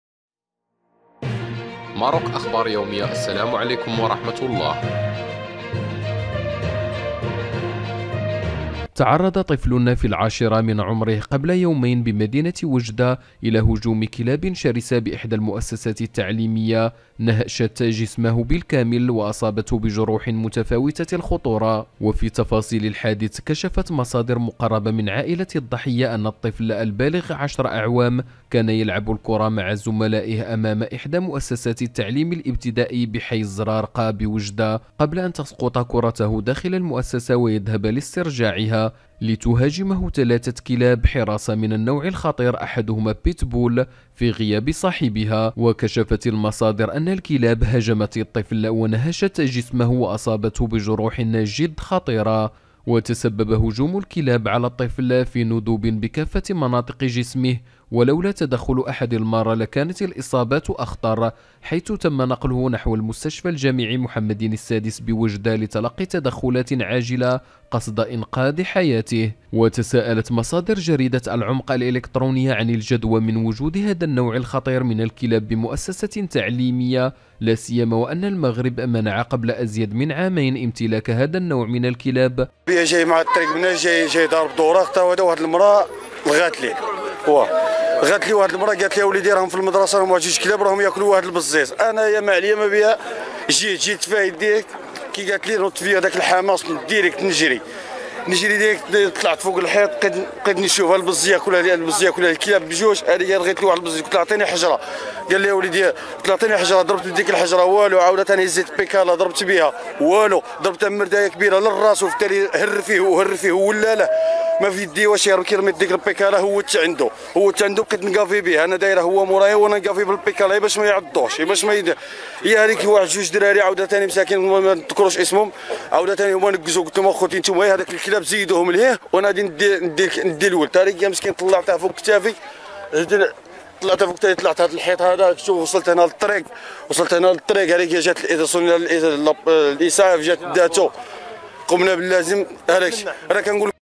شوفو شنو دارو الكلاب لتلميذ وتدخل بطولي من مواطن في وجدة
Artiste : __url_artiste__